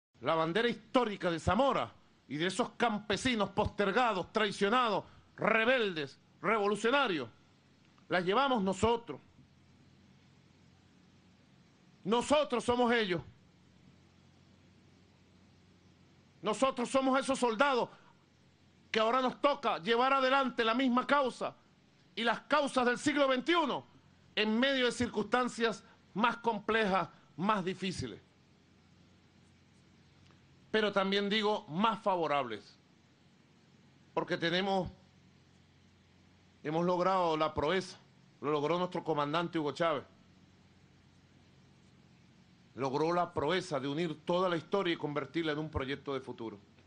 Pdte. Maduro recordó lucha de Ezequiel Zamora en la Guerra Federal
Presidente Nicolás Maduro recuerda la lucha histórica de Zamora durante la Guerra Federal y exhorta al pueblo que se mantenga en pie de lucha